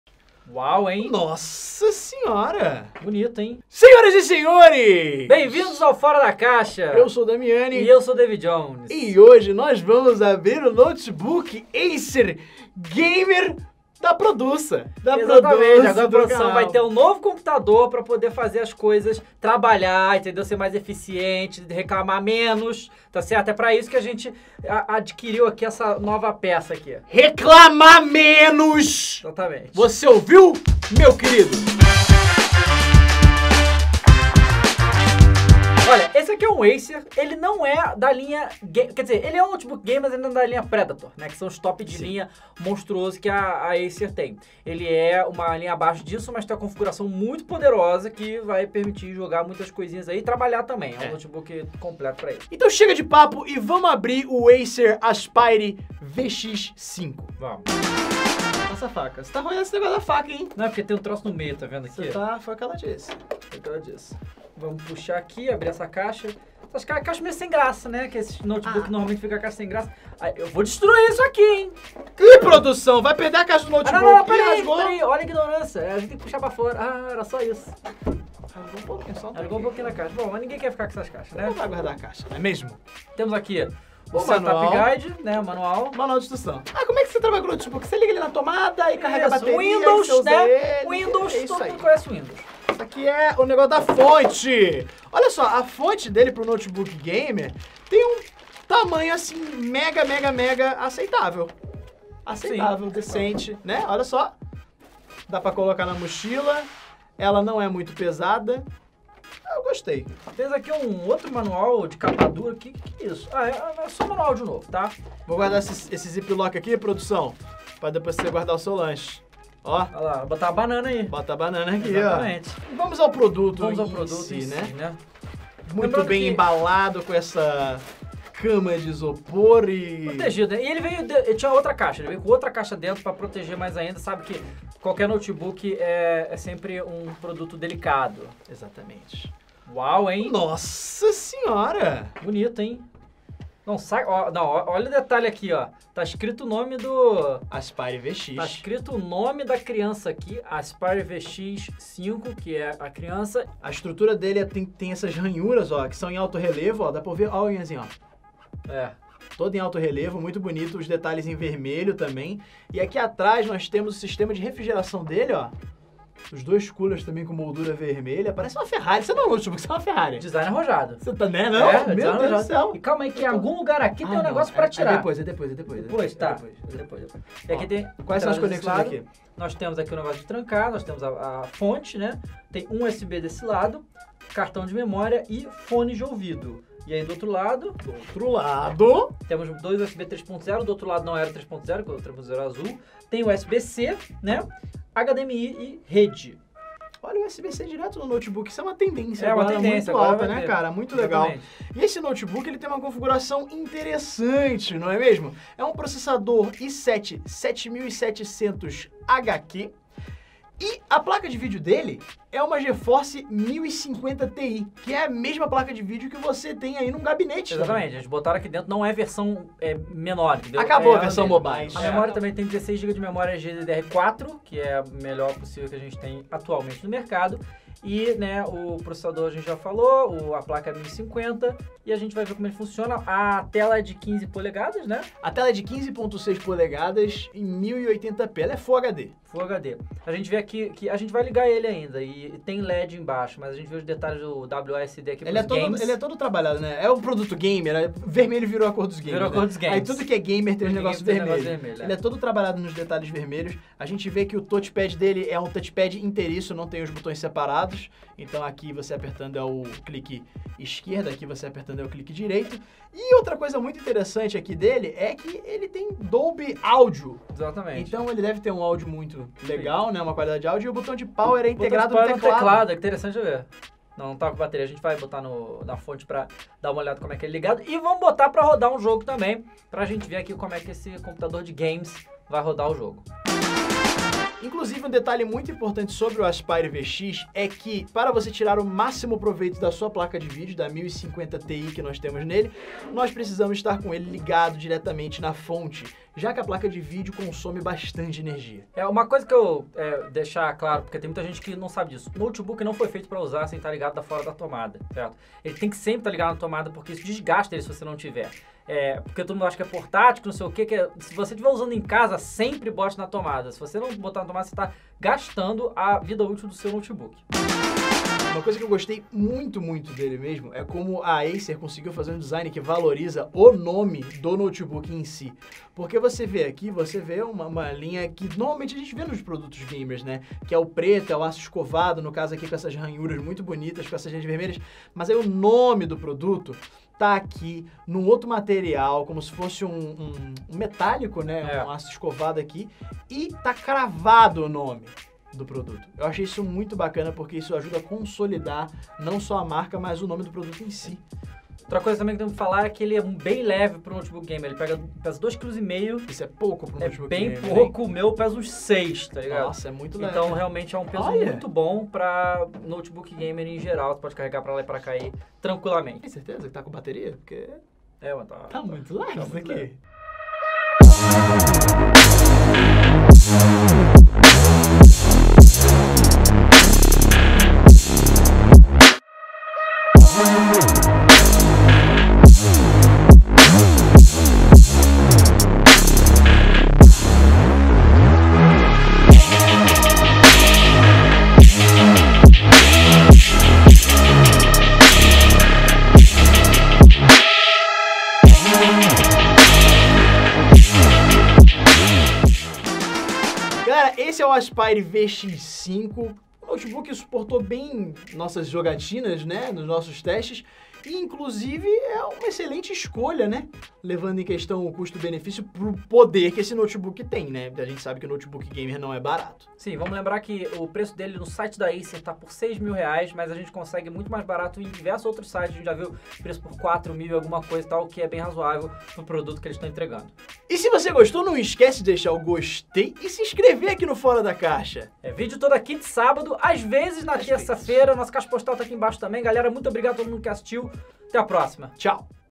Notebook GAMER PODEROSO e muito bonito - Unboxing Notebook Gamer Acer Aspire VX 15
Um notebook gamer poderoso e muito leve! Hoje no Fora da Caixa vamos fazer unboxing do Notebook Gamer Acer Aspire VX 15, uma poderosa máquina equipada com uma Nvidia 1050ti, um processador i7 de sétima geração e 16gb de DDR4, tudo que você precisa para rodar seus games nessa linda máquina!

• MANDA SUA CAIXA QUE A GENTE ABRE EM VÍDEO •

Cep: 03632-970
Sâo Paulo - SP

• Uma Produção Damianizando & Davy Jones •